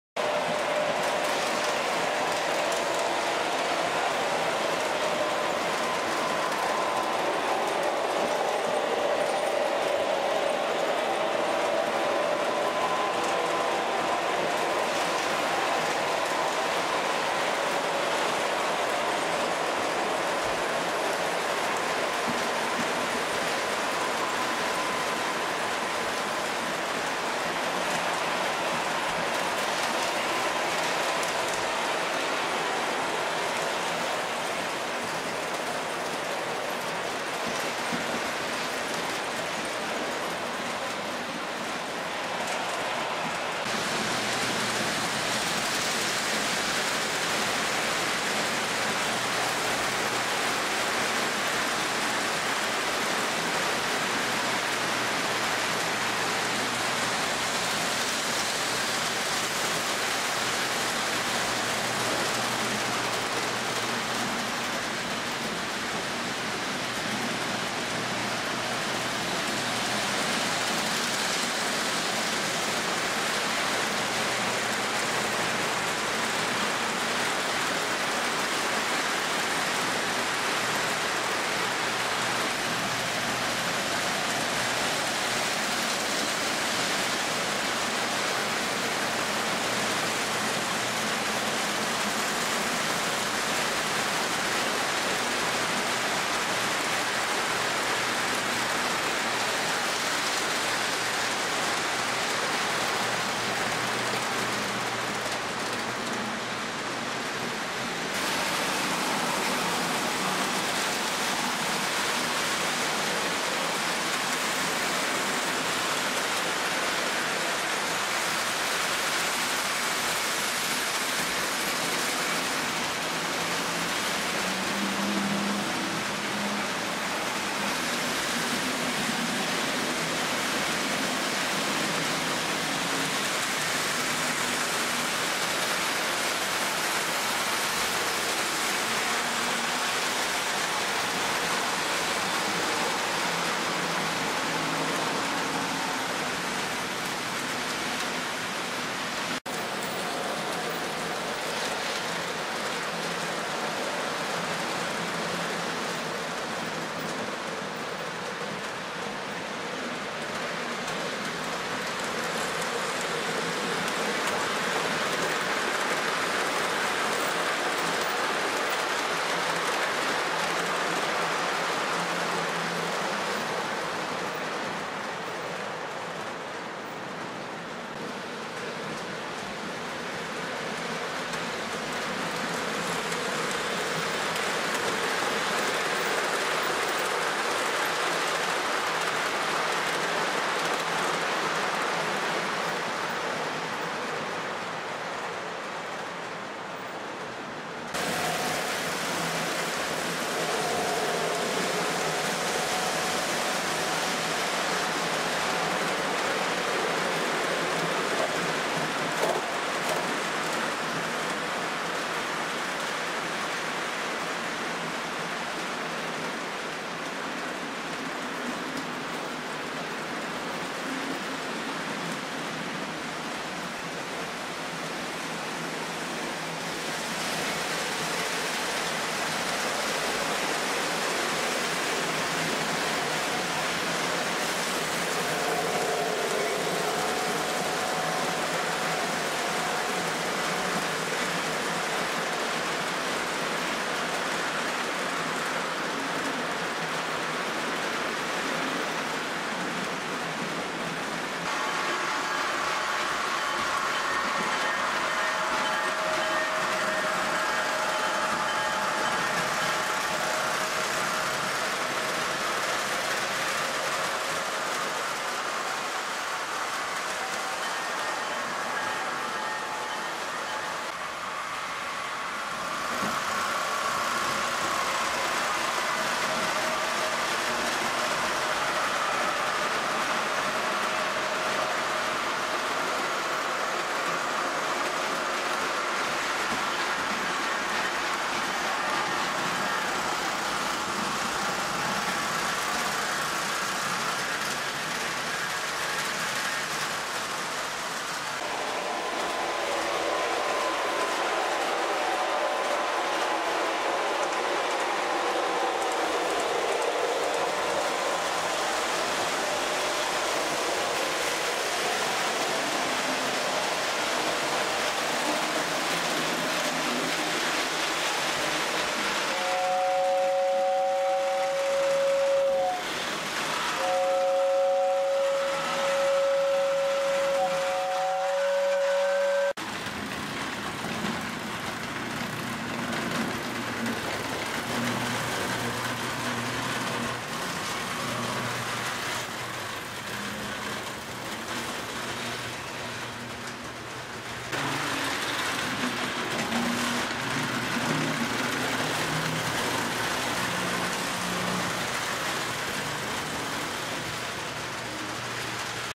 Unit, and solid trains on a Lionel postwar and modern layout
Grain, Ore, wood products, coal, oil, cement, and intermodal unit or solid trains on a postwar Lionel and modern 3 rail layout.